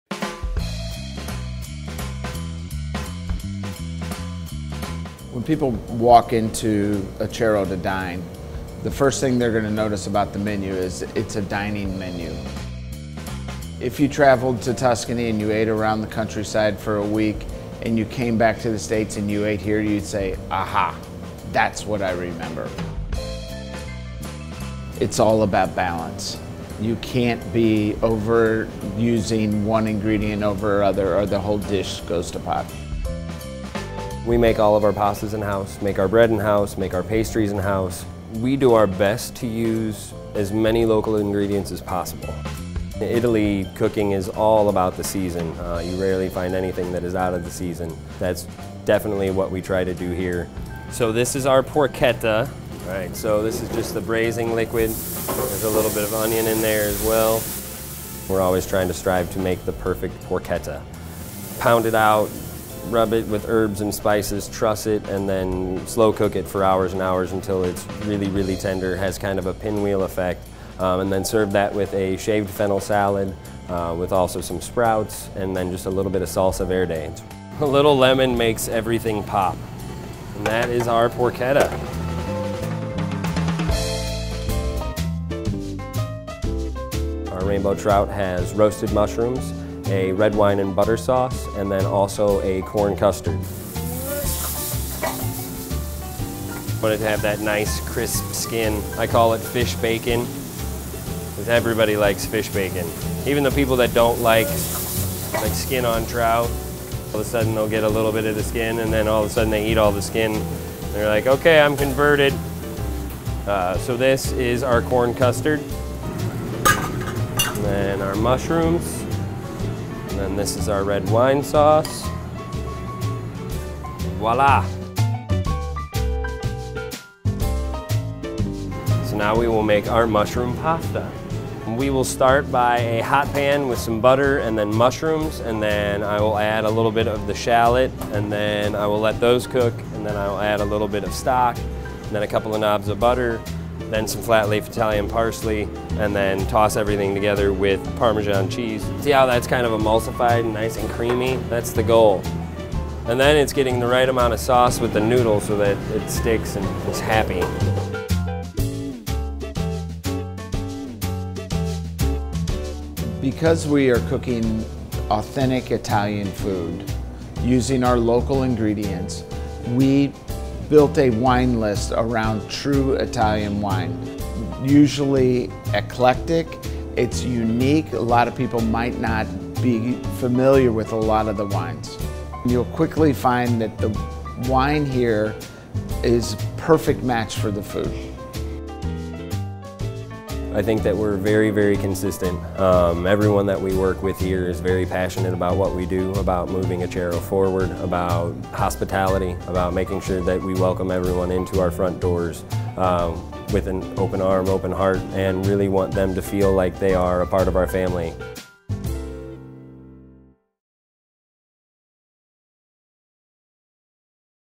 A Taste of Acero
Get a taste of Acero’s Tuscan-inspired menu, featuring scratch-made pastas, breads and pastries.